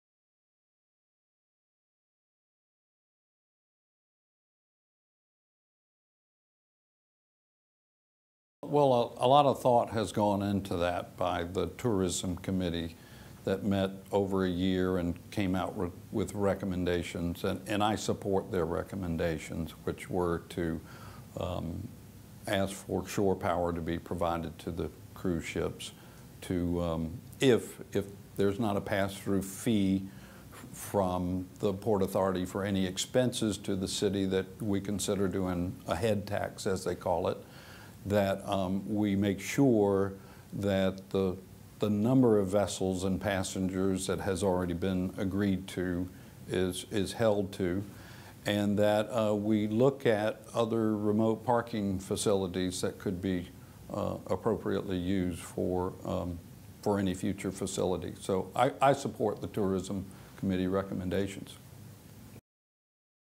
John Tecklenburg on the Cruise Industry
For the first time in 40 years, Charlestonians will elect a new mayor. As an active member of local politics, the Coastal Conservation League invited all of the mayoral candidates to be interviewed in hopes of learning how they plan to incorporate conservation into their work as mayor. We had the pleasure of interviewing Ginny Deerin, William Dudley Gregorie, Toby Smith, John Tecklenburg, Paul Tinkler, and Maurice Washington–all of the candidates except Leon Stavrinakis, who declined our invitation to participate.

Each candidate answered the same set of questions on five topics that relate to the League's work in the City of Charleston. We are presenting the videos of their responses on our website by topic for our membership and the public to view. We hope you enjoy getting to know the candidates better and find the videos helpful in making an informed voting decision.

As a 501(c)(3) nonprofit, the Coastal Conservation League is prohibited from participating in any actions in support of or opposition to candidates. We offered the same opportunity to all of the candidates.